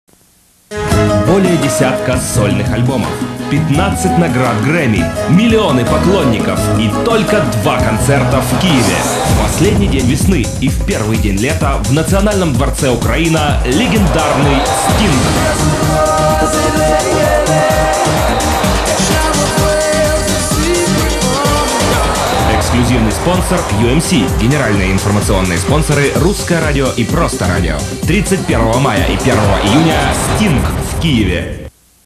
Sting concert promo
I made script, all editing, and motion graphics for this commercial. Also I recorded actor's voice, processed it and mix with music.